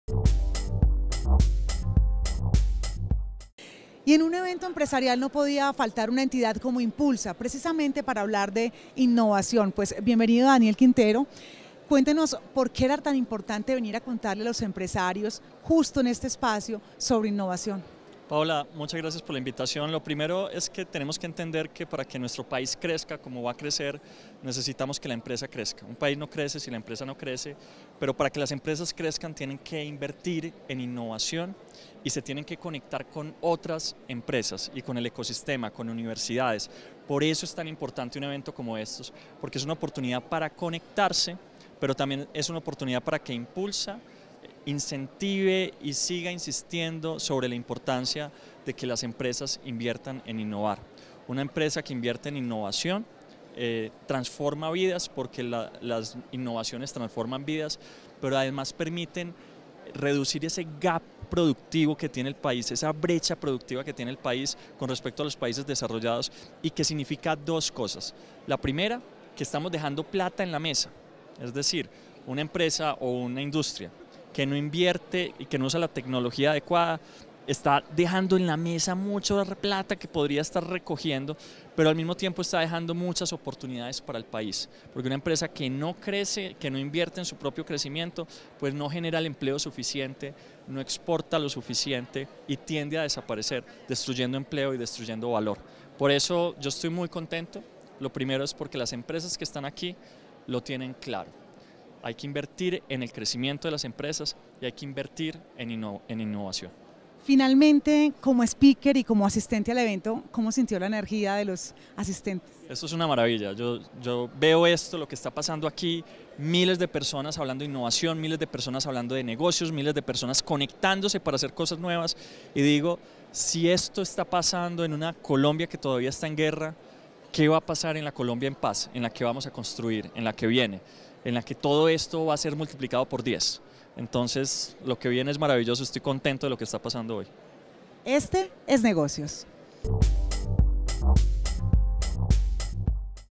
Importancia de invertir en innovación para el desarrollo del país: Daniel Quintero Calle
Daniel Quintero Calle (Twitter: @QuinteroCalle), Gerente general de iNNpulsa Colombia, comparte la importancia del crecimiento de las empresas para el desarrollo del país, mediante acciones y actividades como inversión en innovación e interrelacionamiento con el ecosistema, junto a nuestra subdirectora y presentadora Paola Rueda López (Twitter: @Paolaruedalopez) desde el World Business Forum WOBIBogotá2016.

"Una empresa que no invierte en su crecimiento y que no posee la tecnología adecuada, no genera ingresos suficientes, no genera el empleo que podría generar y está destinada a desaparecer" argumenta Daniel Quintero Calle.